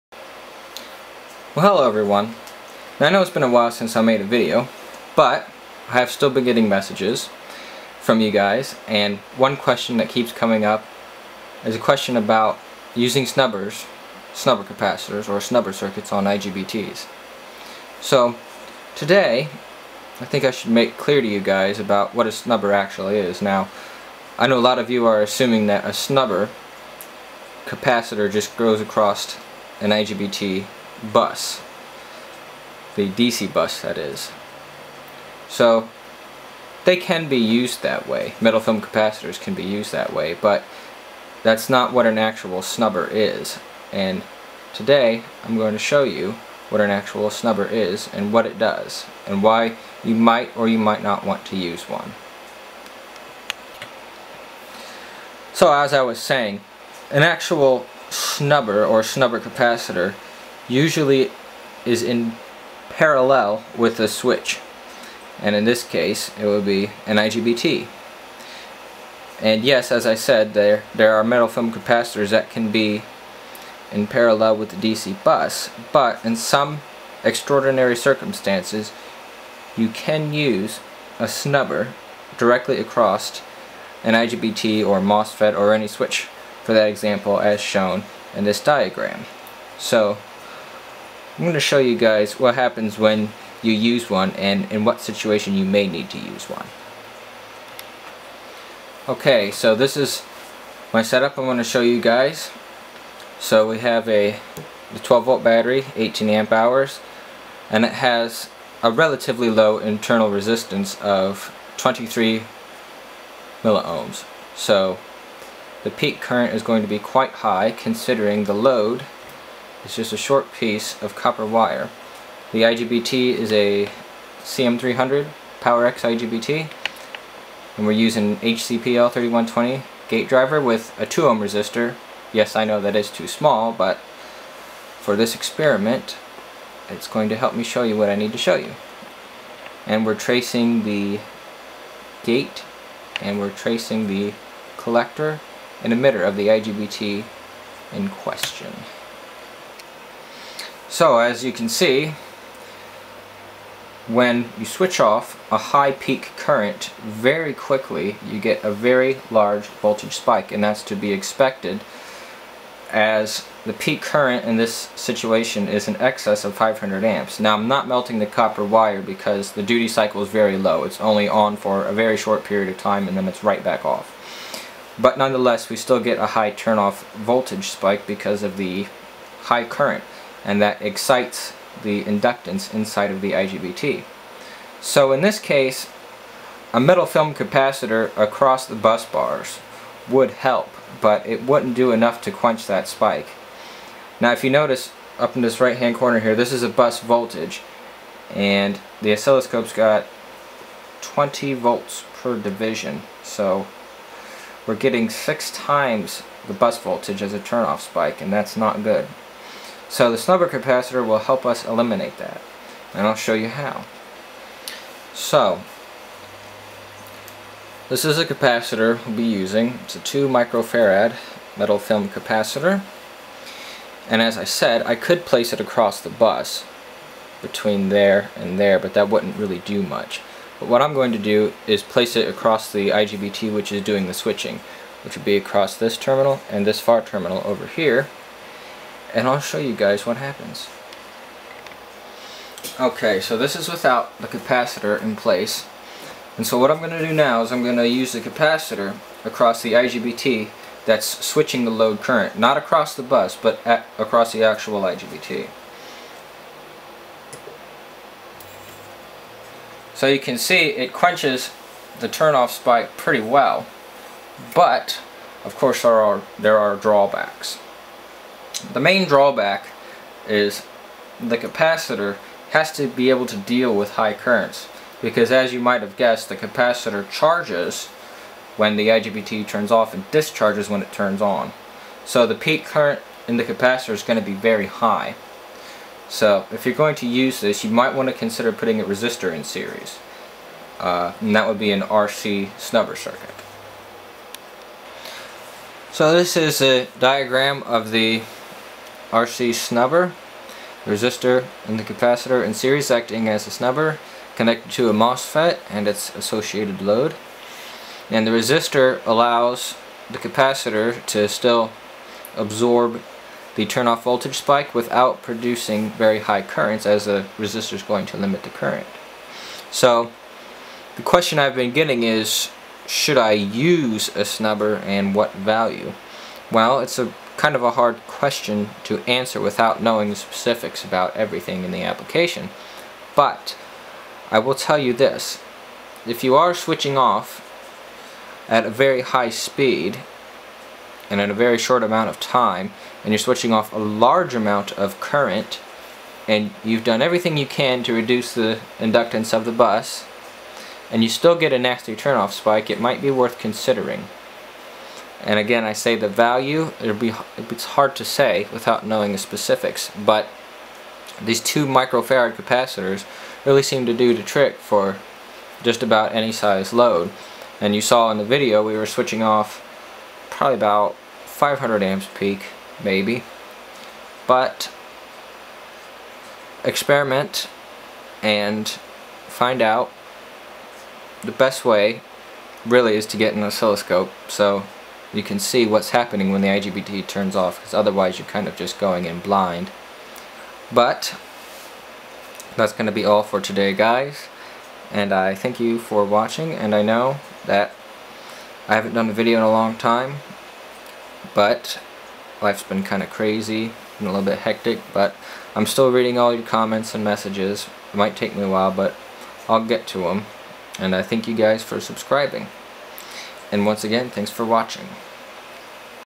Using IGBT Snubber circuits
A while ago I was asked about selecting and using IGBT snubber circuits. In this video I demonstrate what effect a small snubber capacitor has on an IGBT chopper circuit.